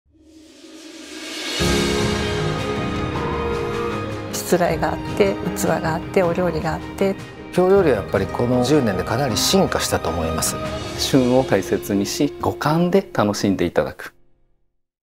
【The Next Future of "Washoku"】Events for experiencing Kyoto cuisine －Short Version－
To celebrate the 10th anniversary of Washoku's inclusion in the UNESCO Intangible Cultural Heritage list, we held events in Kyoto restaurants where you could experience Kyoto cuisine, a registered intangible cultural property, and culture and history that have been developed together. －Short Version－